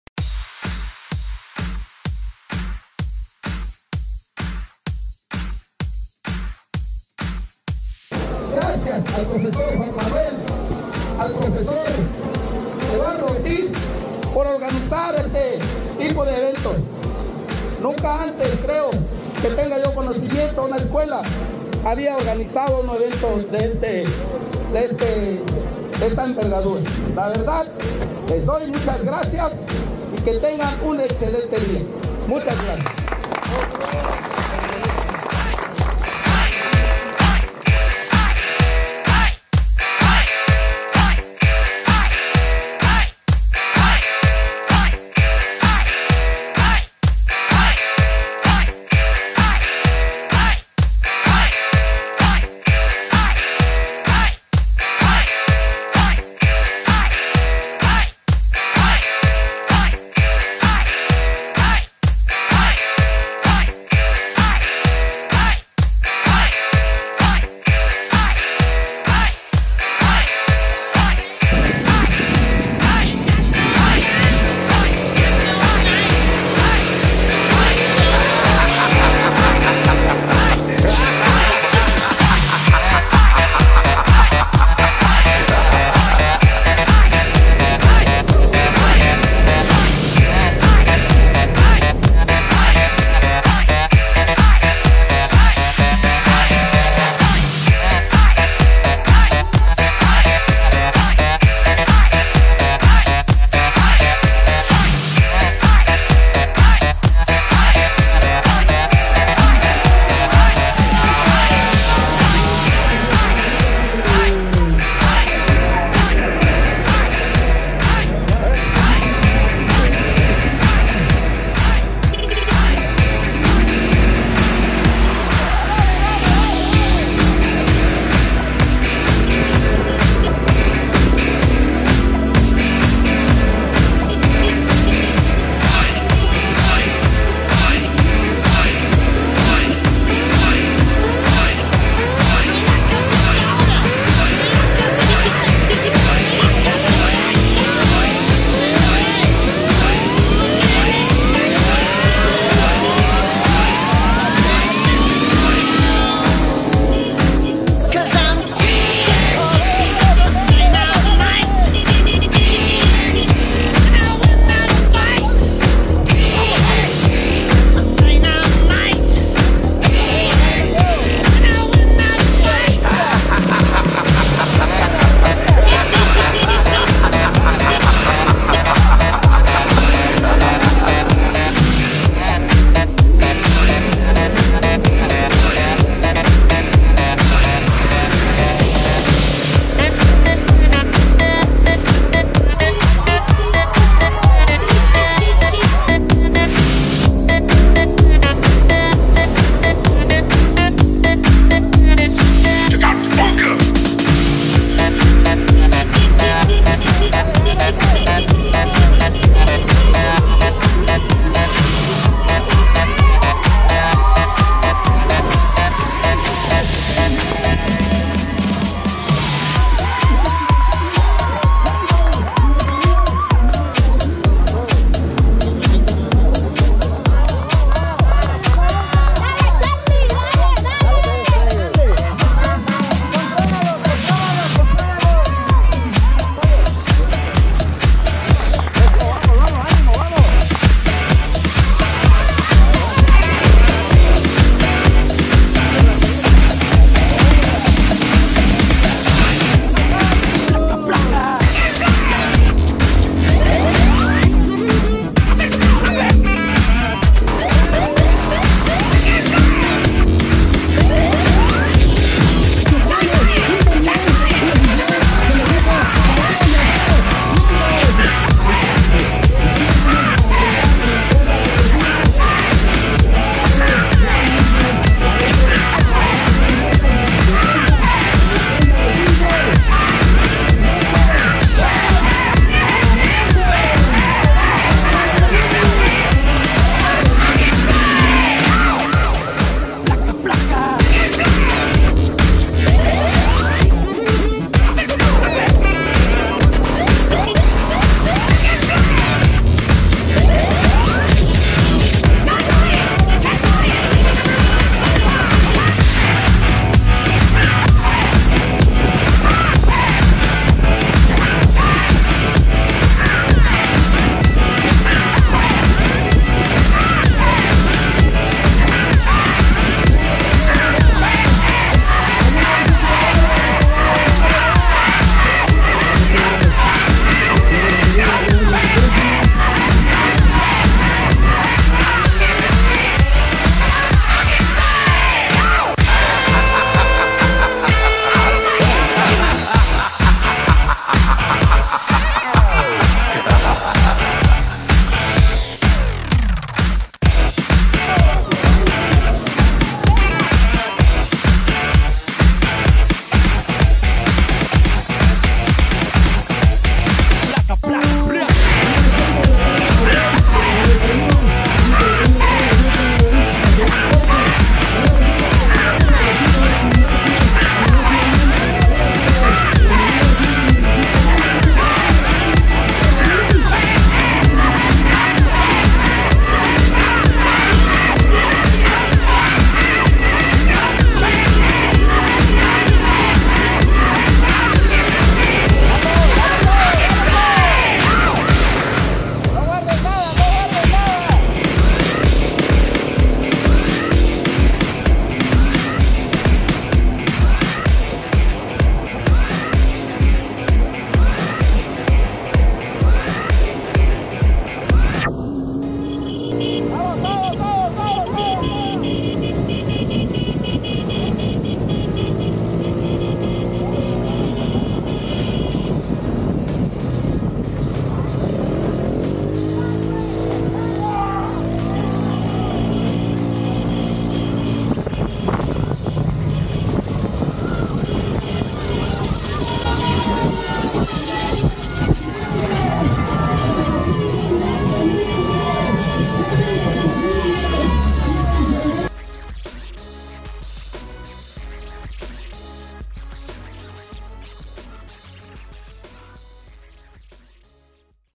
1er Carrera del Circuito Ignacio Allende
Celebrando 86 años de aniversario la Escuela Primaria Ignacio Allende realizo su primer carrera en el circuito de Septune